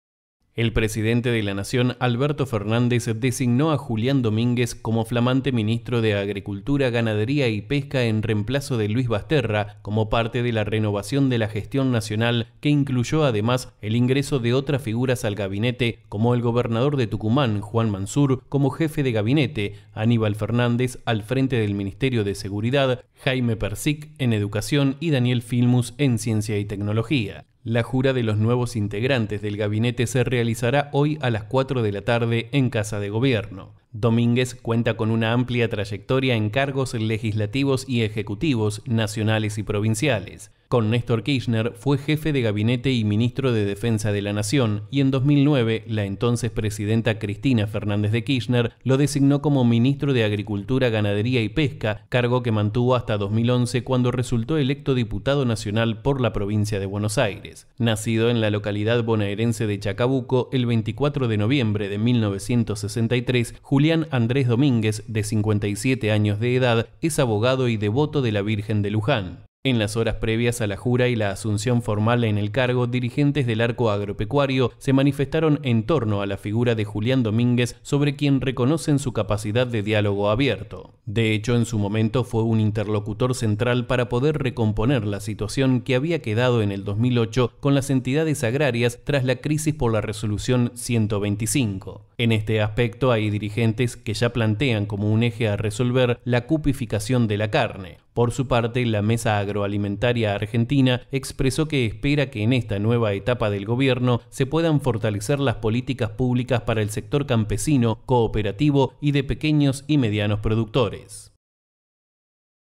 Julián Domínguez, nuevo ministro de Agricultura, Ganadería y Pesca
El Presidente de la Nación renovó parte de su gabinete y nombró al bonaerense Julián Domínguez en reemplazo de Luis Basterra en el ministerio de Agricultura, Ganadería y Pesca.